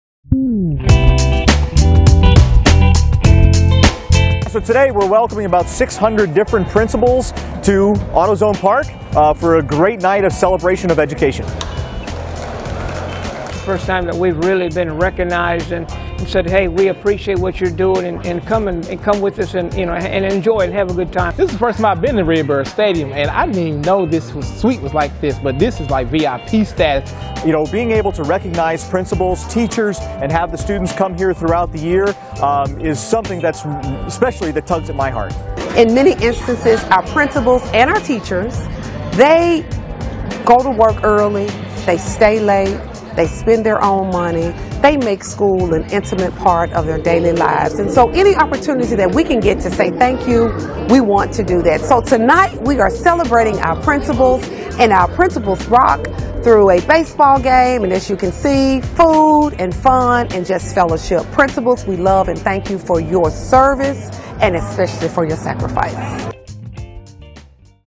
Principals Rock
Shelby County Schools Principals ROCK! Principals from all across the district were shown how much they are appreciated with a night out at AutoZone Park.